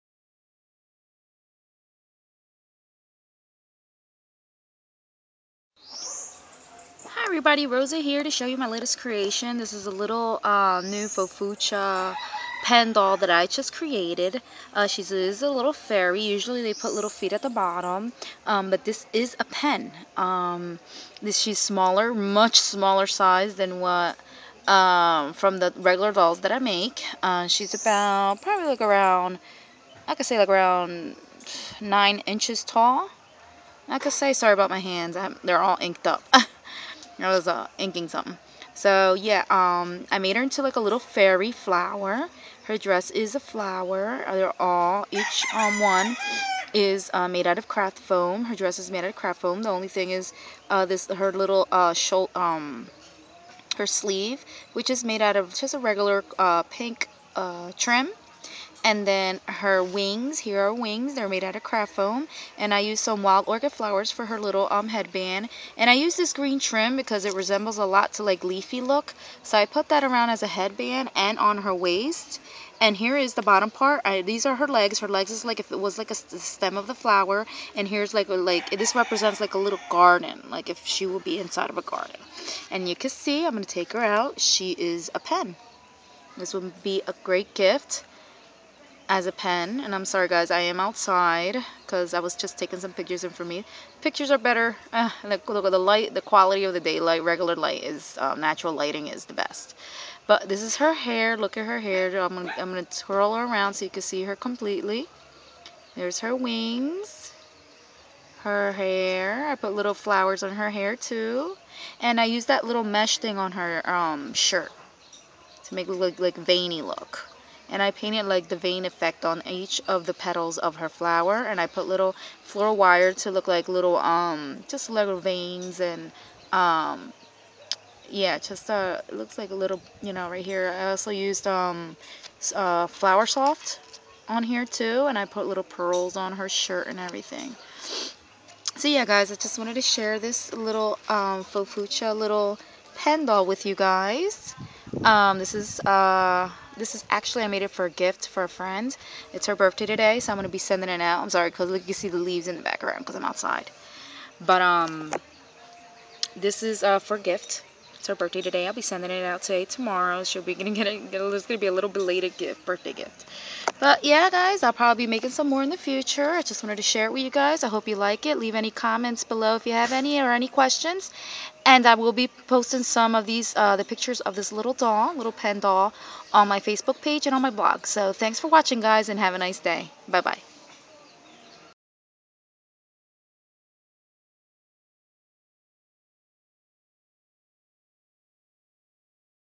Fairy Fofucha Doll Pen Foamy Doll
Hi everyone here is a Fofucha doll Pen that I made, just wanted to share, will have pics in my facebook and blog, TFW!!! Happy Crafting everyone!!